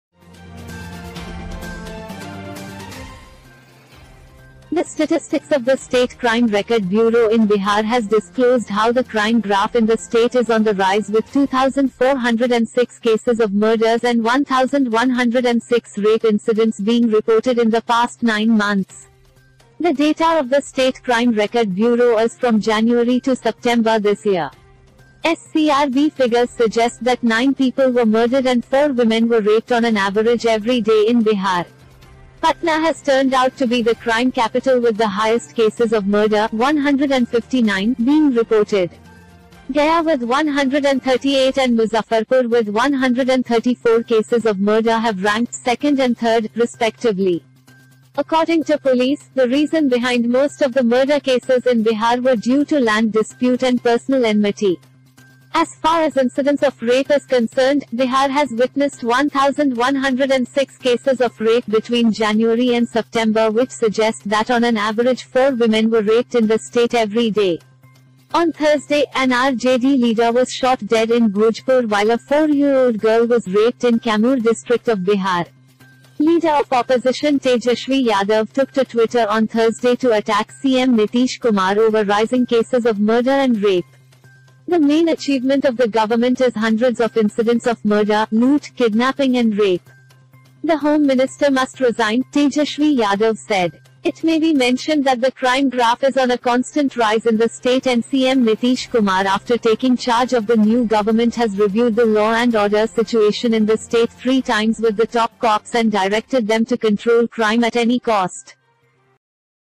Crime out of control in Bihar, 2,406 murders and 1,106 rapes reported in 9 months
0:10 Crime out of control in Bihar, 2,406 murders and 1,106 rapes reported in 9 months